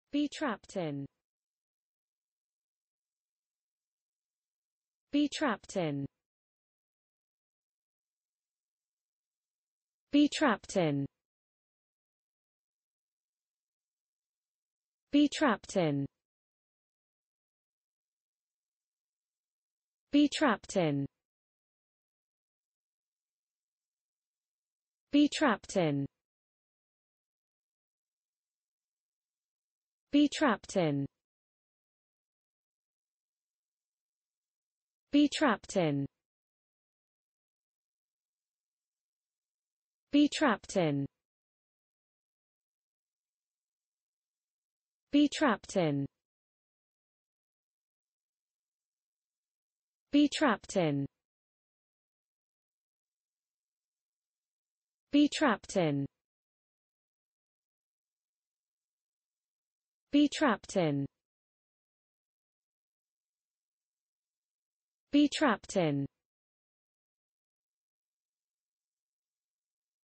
be trapped in: How to pronounce be trapped in in english (correct!).Start with B. Learn from me.
Follow me!Learn from me how to pronounce the word be trapped in in english.This video shows you how to pronounce the word be trapped in in english.

be trapped in

被困在